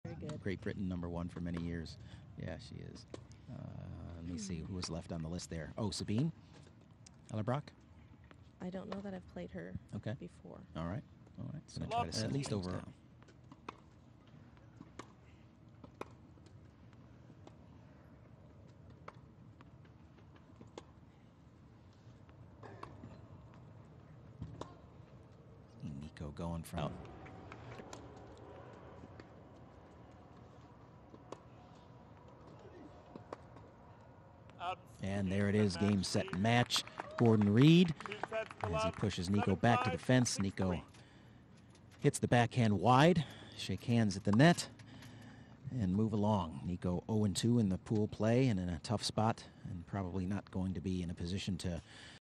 Highlights : Reid vs Peifer - NEC Wheelchair Tennis Masters 2018 | FFT
Revivez les meilleurs moments du match entre Gordon Reid et Nicolas Peifer au NEC Wheelchair Tennis Masters 2018.